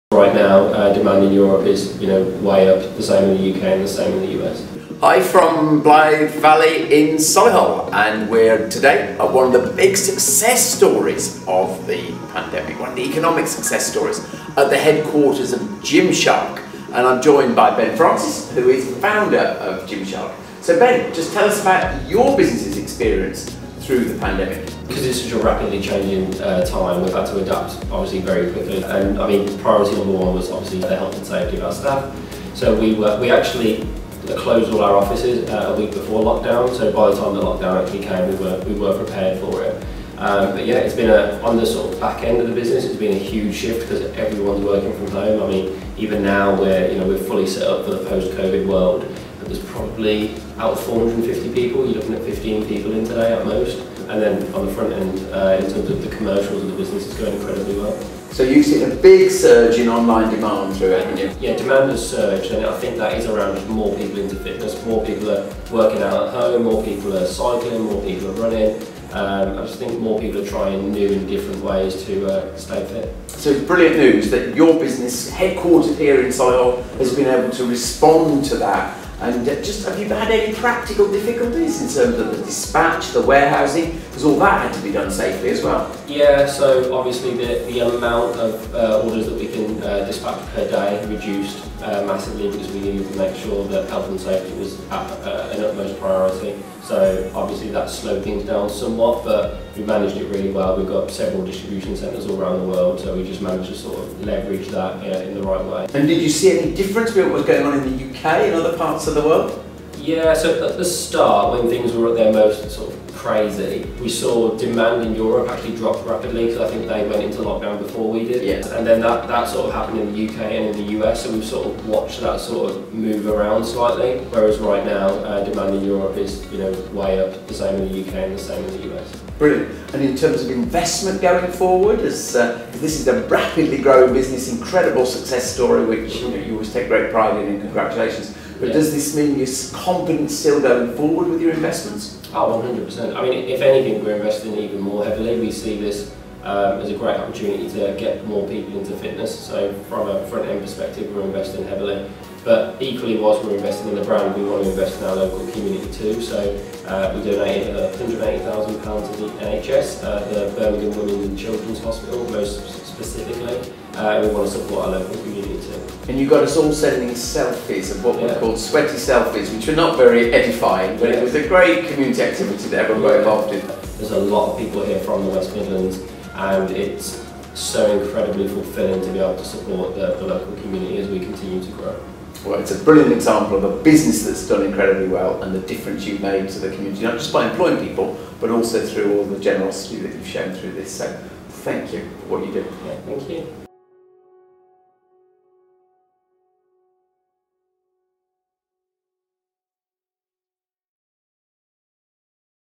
Andy Street and Ben Francis - Gymshark Visit
A WEST MIDLANDS UNICORN 🦄
Recently, I caught up with Ben Francis at Gymshark HQ to see how his now, £1bn business, fared during the pandemic.